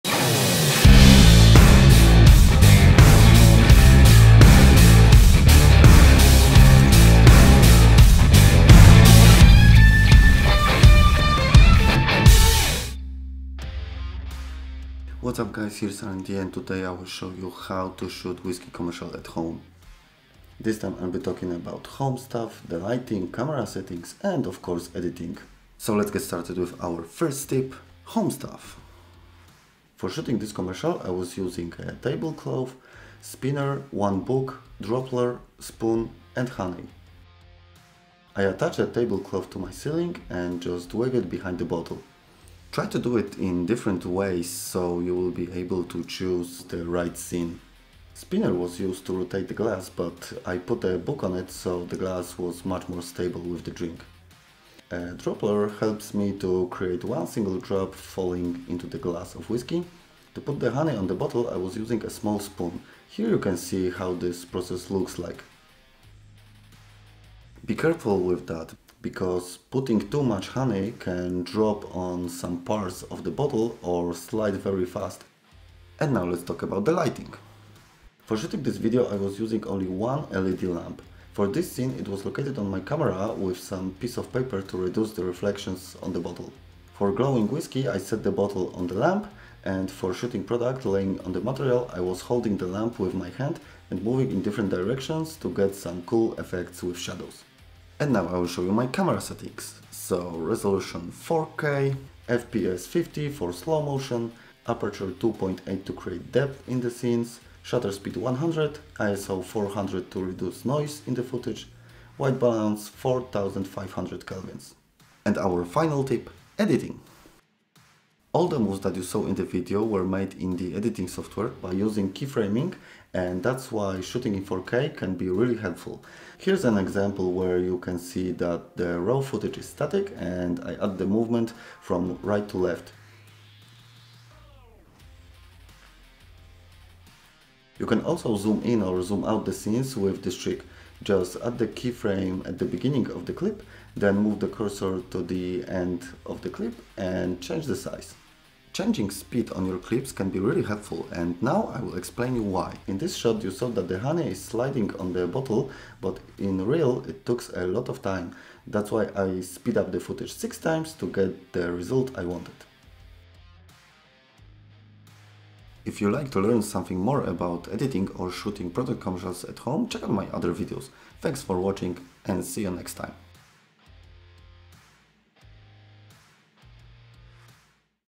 Shooting Whisky Commercial Jim Beam Honey at Home
How to shoot product commercials at home? This time I'll show you how to record epic whiskey Jim Beam Honey in low budget.
My TOP TIPS: Home stuff, lighting, camera settings, and editing.

Product videos are now the most popular, because companies want to show their products in web and the videographers started the race who will create better one or have the best ideas.

Camera: Lumix GH5
Lens: Lumix G Lens 30mm F2.8
Tripod: Velbon

Timestamps:
00:00 Intro
00:26 Home Stuff
01:16 Lighting
01:38 Camera Settings
01:56 Editing